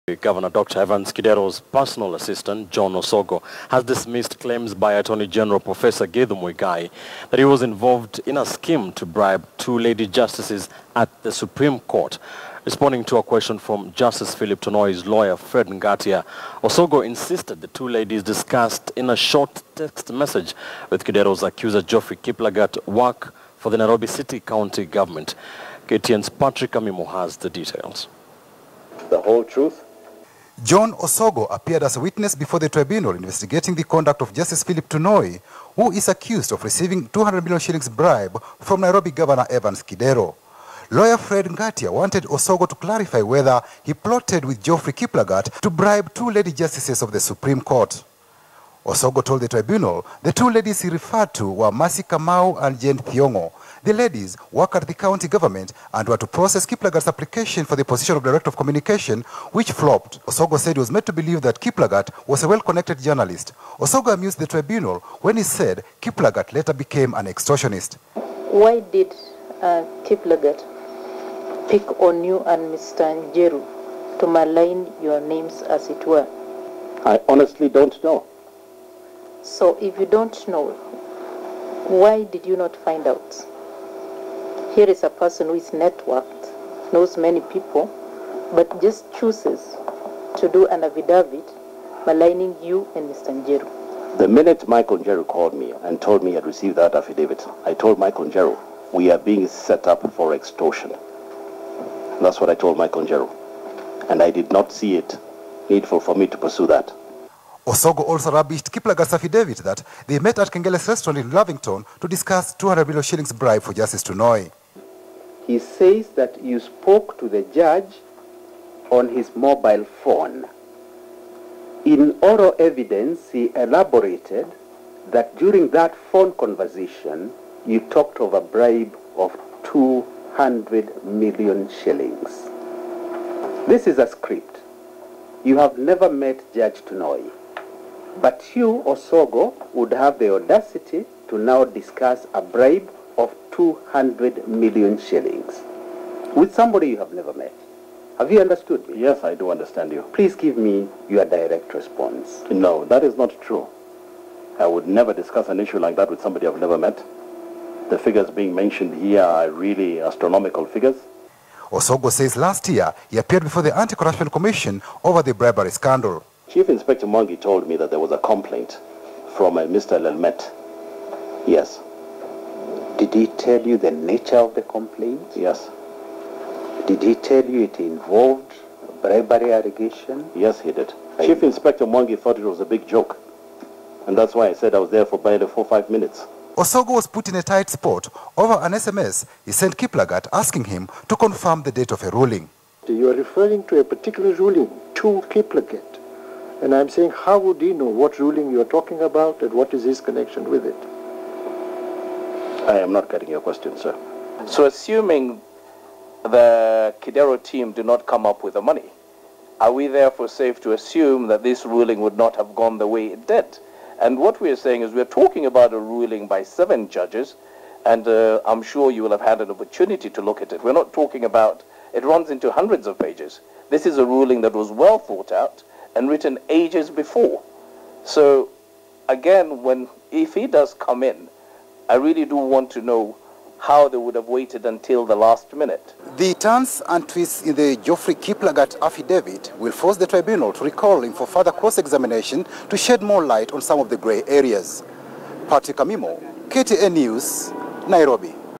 Governor Evans Kidero’s PA questioned by tribunal investigation Justice Philip Tonui
Nairobi Governor Dr. Evans Kidero’s personal assistant John Osogo has dismissed claims by attorney general professor Githu Muigai that he was involved in a scheme to bribe two lady justices at the Supreme Court.  Responding to a question from Justice Philip Tonui’s lawyer Fred Ngatia, Osogo insisted the two ladies discussed in a short text message with Kidero’s accuser Geoffrey Kiplagat work for Nairobi city county government.